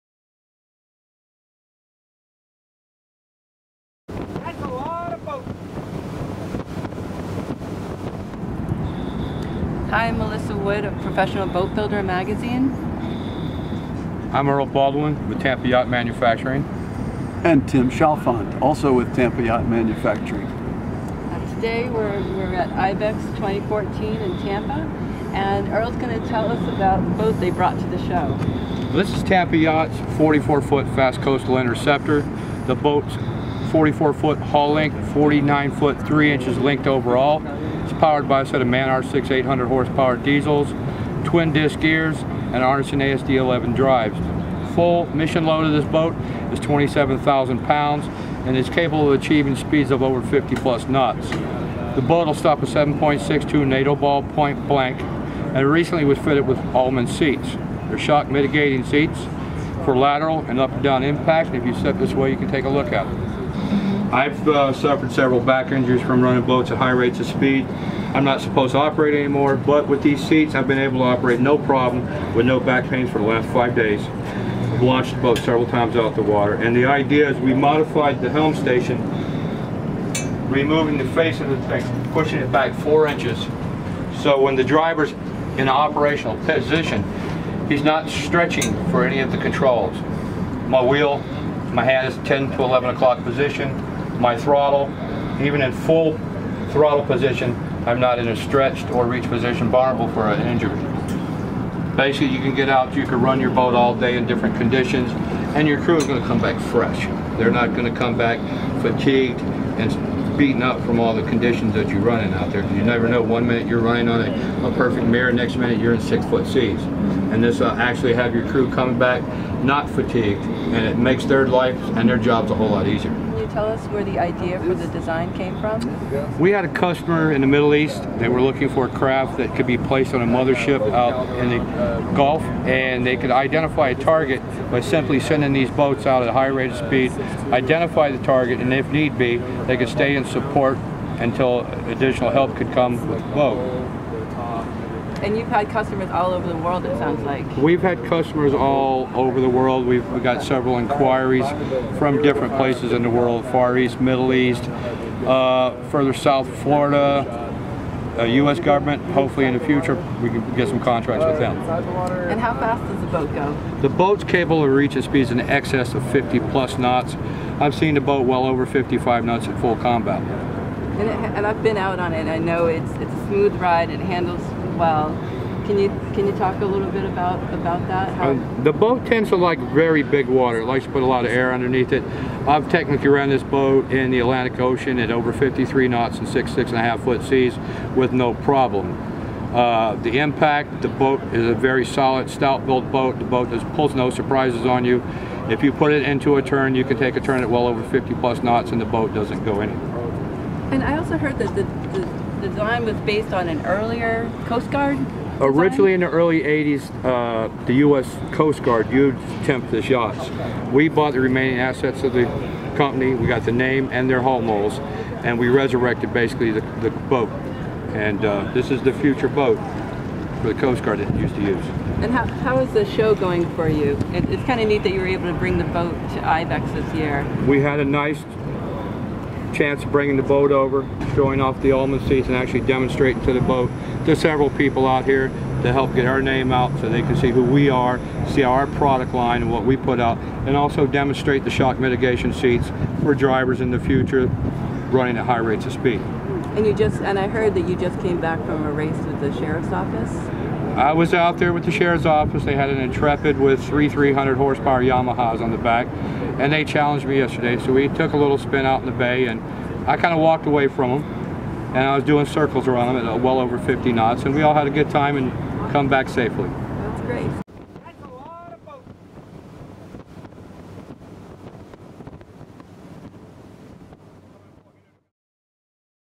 Fast Coastal Interceptor -Tempest The Tempest 44-FCI
The Tempest 44-FCI, Fast Coastal Interceptor is a state-of-the-art high-speed coastal interceptor boat.  It was first introduced in 2006 by Tampa Yacht Manufacturing in the United States.  The 44-FCI is a 44 ft. (13.2 m) interceptor-class vessel with a proven design heritage that has seen historic action with US Customs and the US Coast Guard for more than two decades.

A totally redesigned world class naval vessel, the 44-FCI is fully customizable with featured enhancements and modifications that include, but are not limited to, 360 degree pan and tilt infra-red night vision camera, 50-caliber machinegun mounts and threat level NIJ-III and IIIA ballistic protection, utilizing  Dyneema tm and Spectra tm  HDPE fiber.

Designed for coastal and offshore operations in varying sea and weather conditions, the 44-FCI is capable of achieving a maximum speed of 55 knots (WOT) with a minimum sustained cruising speed of 38 knots. With a minimum range of 225 nautical miles at wide open throttle and a cruising range of 300 nautical miles, the fully-outfitted 44-FCI is a very capable and effective intercepting vessel of unmatched performance in its class.

SPECIFICATIONS
LENGTH HULL (Molded)     43' 2-1/4" (13.16 m)
LOA (Max)                            47' 3-1/2" (14.41 m)
BEAM SHEAR MAX             10' 8-3/4" (3.35 m)
DEADRISE                           24.5 degrees
WEIGHT (Lightship)             23,891 lbs (10,837kg)
DRAFT                                 3' 1-1/2" (0.95 m)
STD ENGINE                       T MAN R6-800
STD HP                                1600 hp (1193 kw)
FUEL CAPACITY                 400 Gal. (1514 L)
PERSONS CAPACITY        4-6 Dedicated Seats
SPEED, FULL LOAD           55.0 kts (63.29 mph)
RANGE, FULL LOAD          300 nm at 38 kts